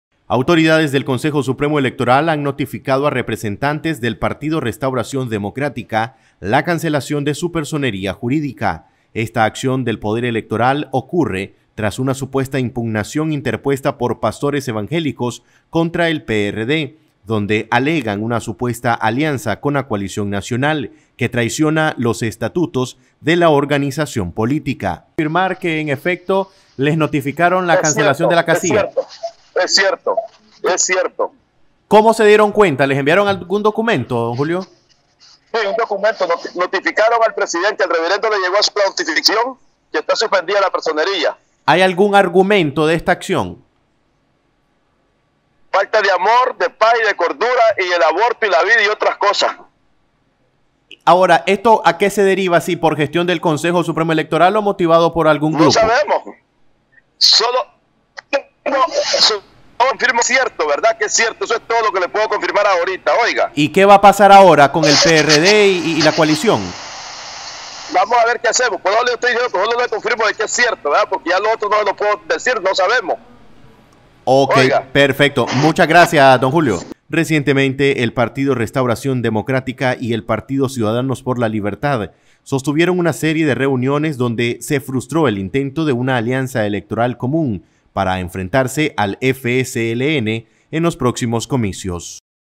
Coalición Nacional se queda sin casilla tras cancelación de personería jurídica al PRD
NosResistimosAlEmbargo Coalición Nacional se queda sin casilla tras cancelación de personería jurídica al PRD.